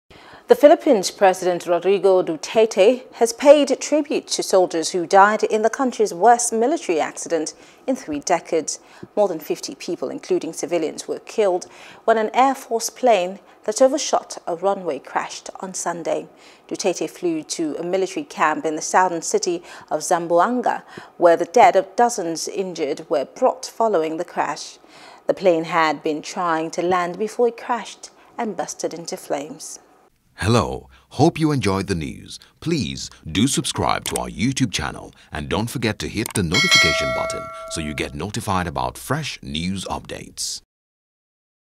Duterte Pays Tribute To Soldiers Killed In Plane Crash | FOREIGN
The Philippine's President Rodrigo Duterte has paid tribute to soldiers who died in the country's worst military accident in three decades.